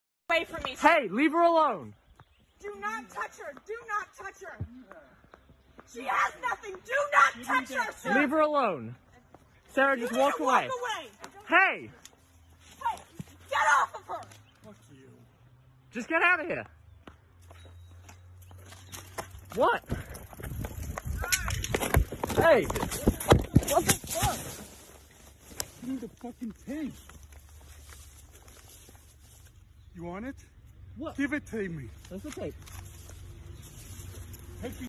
Maryland Protest - Anthony Brennan III : The White man attacks teenage protesters
Police arrest Anthony Brennan III on assault charges after viral video of attack on teenage protesters.

On Friday, The Washington Post reported that authorities have arrested Anthony B. Brennan III, a man suspected of being the attacker caught on camera in Maryland attacking teenagers putting up posters protesting the death of George Floyd, and charged him with second-degree assault.

“One of the three recorded part of the ensuing encounter, which exploded across the Internet on Thursday as social media users spent two days trying to learn the man’s identity,” said the report. “Two men, including a retired police officer, were publicly named by Twitter users as the assailant — even though authorities would later say they had nothing to do with the incident.”

It was not until after 9 p.m. Friday that police announced the arrest of, Brennan, shortly after he turned himself in at the Montgomery County Detention Center in Rockville,” said the report. “Based on the nature of the charges, he was expected to be released from custody on little or no bond.”

In the video, the man, after encountering the trio on the Capital Crescent Trail, started ripping fliers out of the two girls’ hands, allegedly shouting, “You guys are awful. You’ll never amount to anything” and accusing them of inciting riots.

“Do not touch her!” one of them could be heard yelling in the video. After this, he rounded on the third, male youth and pinned him to the ground with his bicycle.